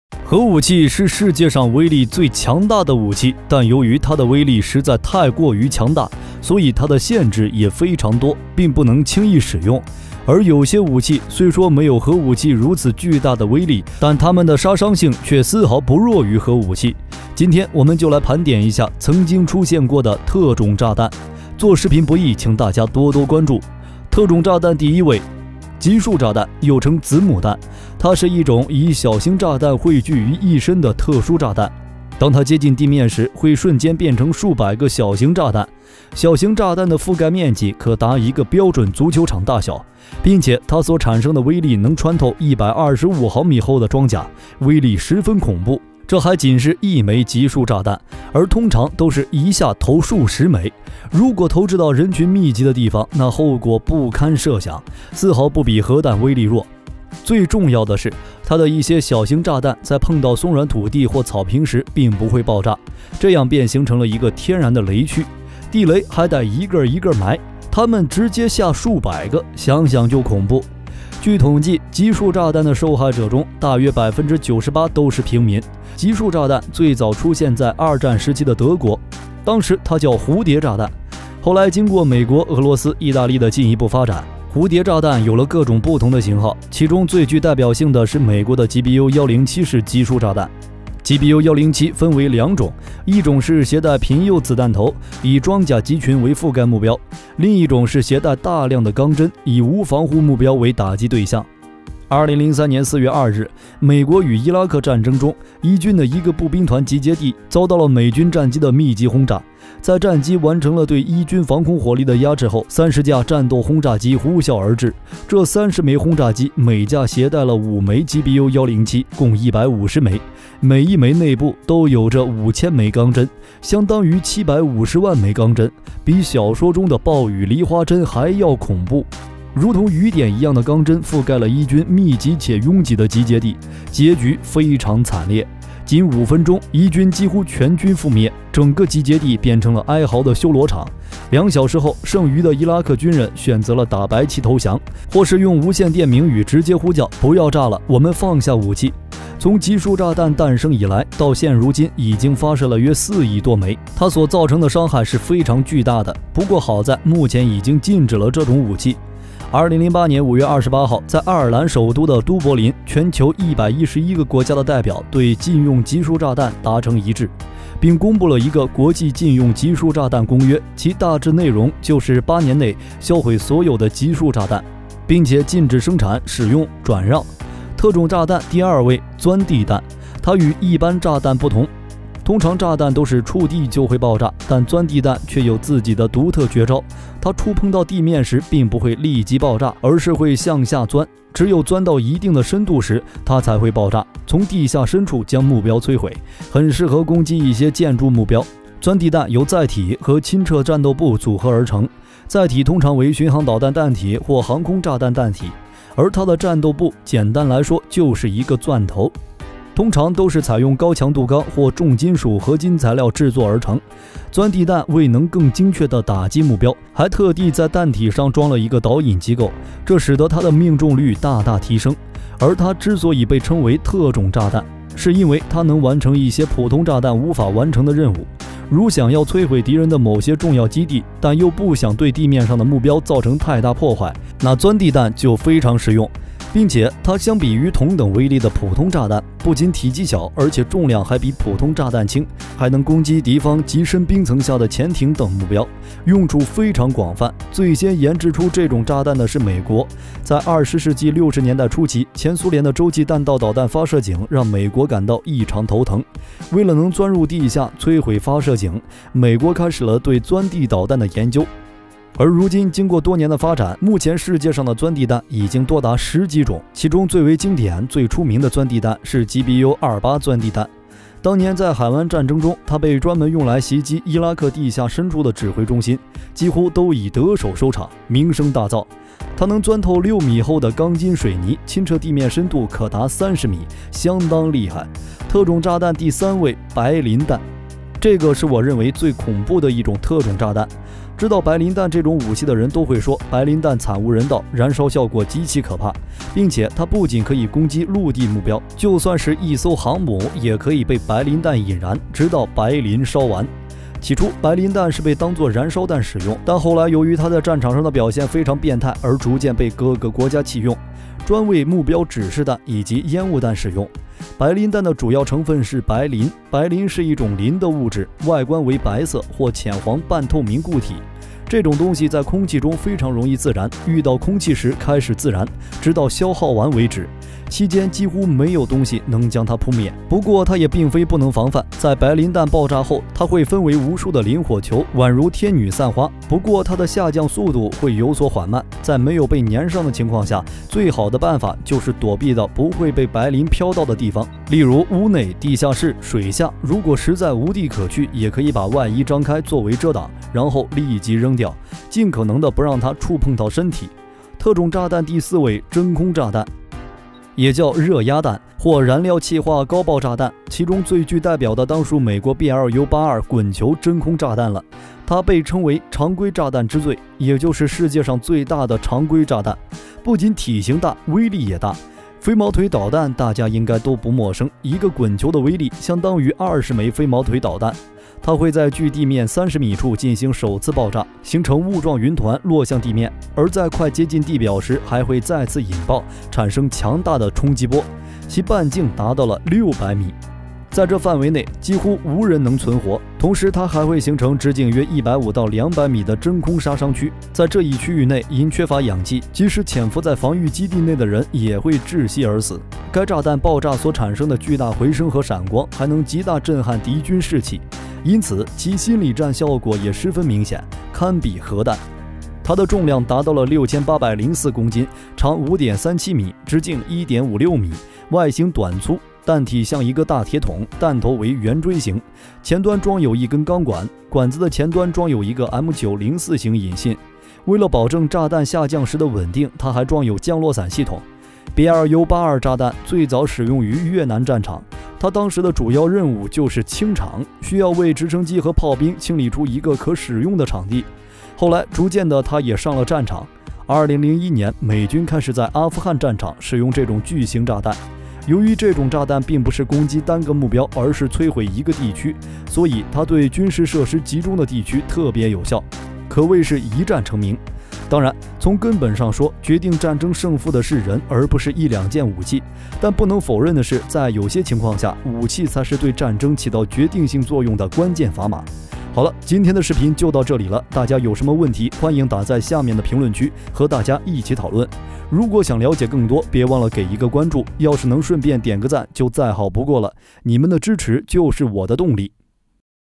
【BLU-82“滾球”】世界現役最大常規炸彈，一顆頂20枚飛毛腿。暴雨梨花針，威力堪稱核彈。史上最恐怖反人類武器，瞬間穿透60米鋼筋混泥土
核武器是世界上威力最強大的武器但由於它的威力實在太過於強大所以它的限制也非常多並不能輕易使用而有些武器雖說沒有核武器如此巨大的威力但它們的殺傷性卻絲毫不弱於核武器今天我們就來盤點一下，集束炸彈（GBU107）、鑽地彈（GBU-28）、白磷彈（MK-77）、真空炸彈（BLU-82“滾球”）
Nuclear weapons are the most powerful weapons in the world, but because of its power is too powerful, it is much more powerful, and some weapons do not use the weapons, although there is no nuclear weapon such a huge power but their killing is not weak. Nuclear weapons today will come to count, bomb bombs (GBU107), drilling (GBU-28), white phosphorus bomb (MK-77), vacuum bomb (BLU-82 "Rolling")